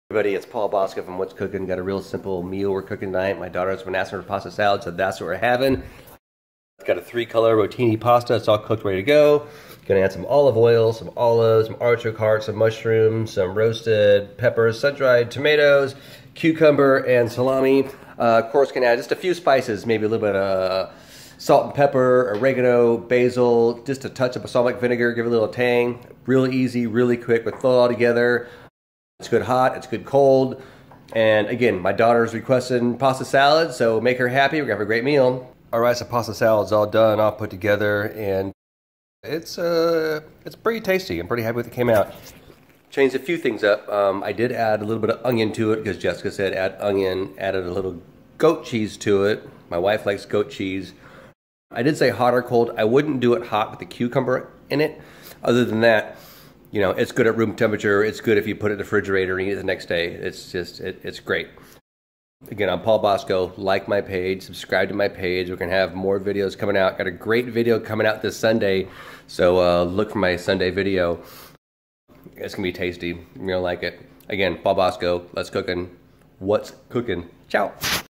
A quick and easy past salad recipie
A short mid-week cooking video.  My daughter asked for pasta salad, so that's what we are having.  Great at room temperature or out of the refrigerator the next day.  Enjoy!